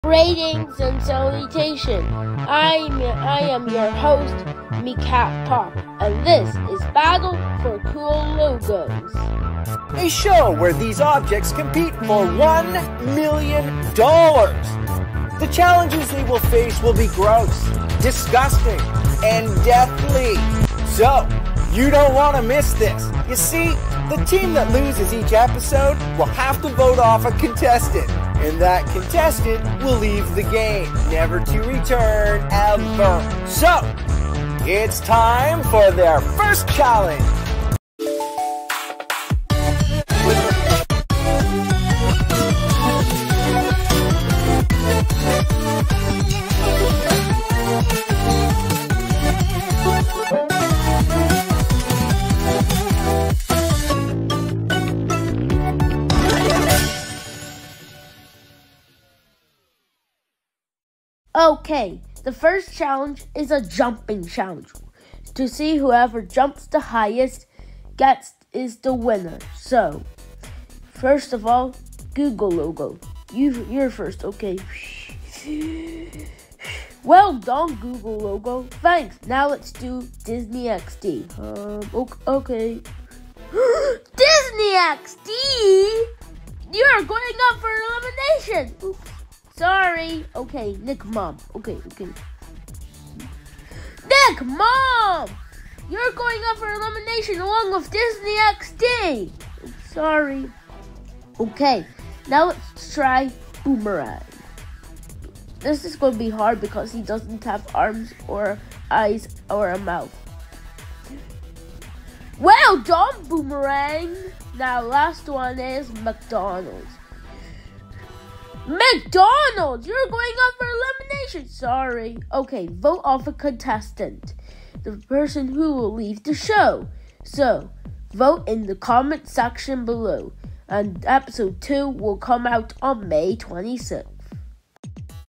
BFCL 1: Jumping Challenge
Contestants Has To Jump As High As They Can To Win.